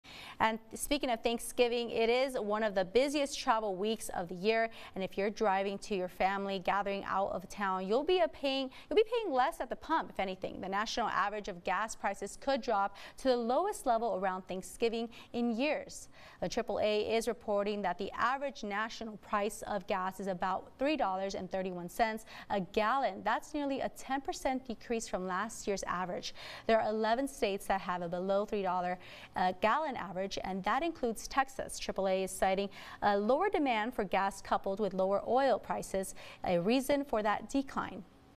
Possible drop in gas prices this year
With Thanksgiving being the busiest travel time of the year, this year gas prices could drop.